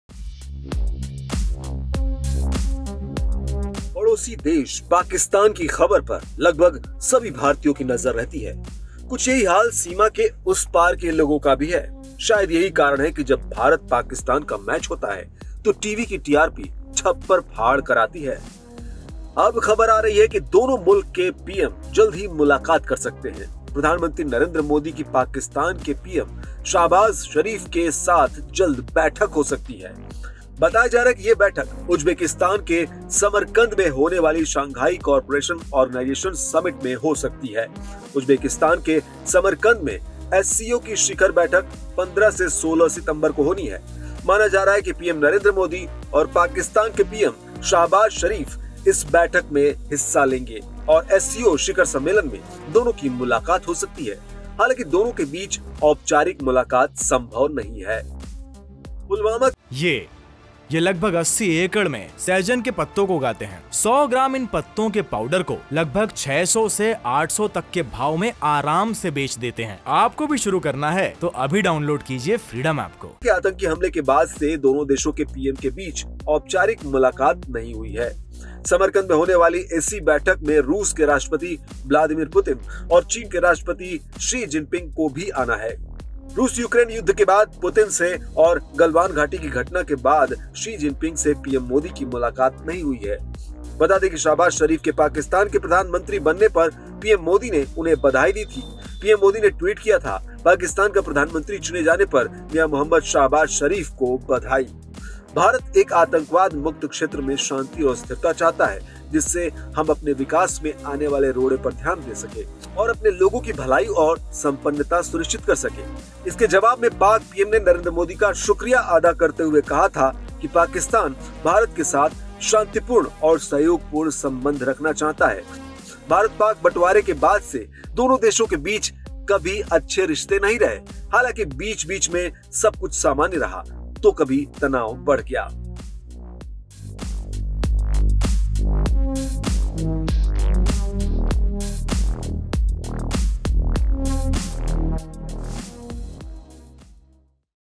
Modi Shehbaz Meeting Pakistan के PM Shehbaz Sharif से मिल सकते हैं PM Modi SCO summit
Modi Shehbaz Meeting: पड़ोसी देश पाकिस्तान की खबर पर लगभग सभी भारतीयों की नजर रहती है...कुछ यही हाल सीमा के उस पार को लोगों का है...शायद यही कारण है कि जब भारत पाकिस्तान का मैच होता है तो टीवी की टीआरपी छप्पर फाड़ कर आती है...अब खबर आ रही है कि दोनों मुल्क के पीएम जल्द ही मुलाकात कर सकते हैं.